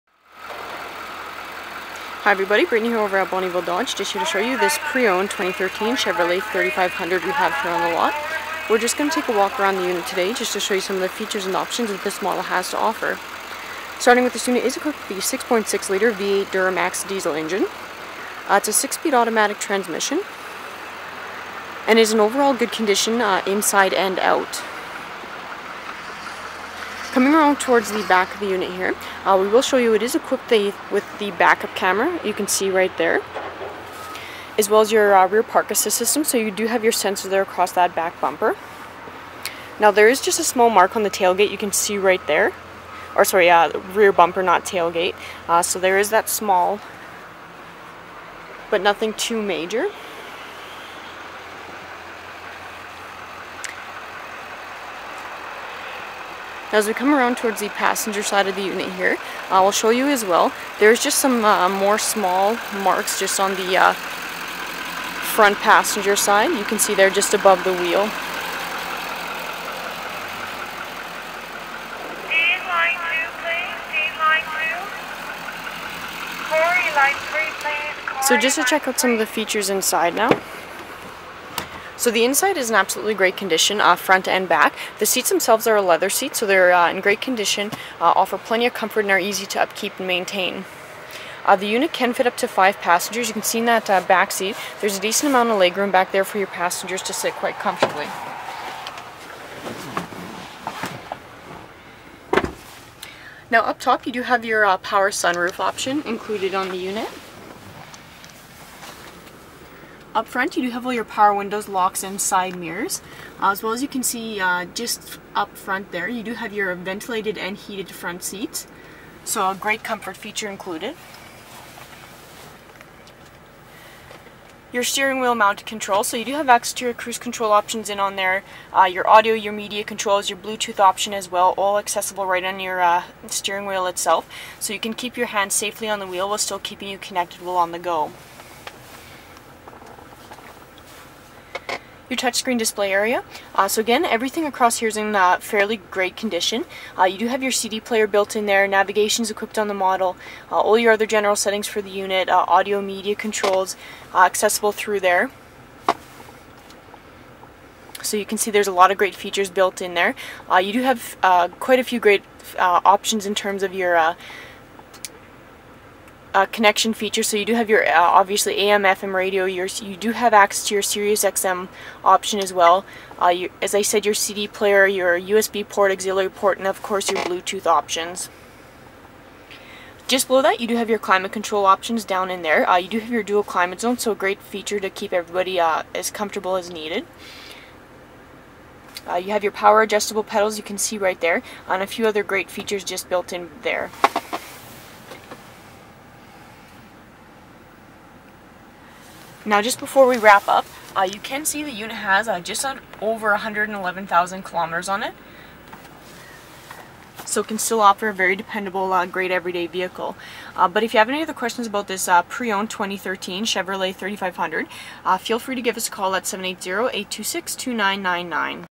2013 Chevrolet Silverado 3500HD LTZ
Backup camera, leather, sunroof, bluetooth are just a small list of the amazing features included inside this 2013 Chevrolet Silverado. Meaning you get plenty of options to keep you comfortable and entertained while on the go no matter how long the journey. Plus you know you can count on the dependability of the ride with 4x4 capabilities, incredible cargo space, towing and hauling option and all the power you could ever need while on the go. So if you need a vehicle to offer you plenty of versatility this truck could be the perfect option for you.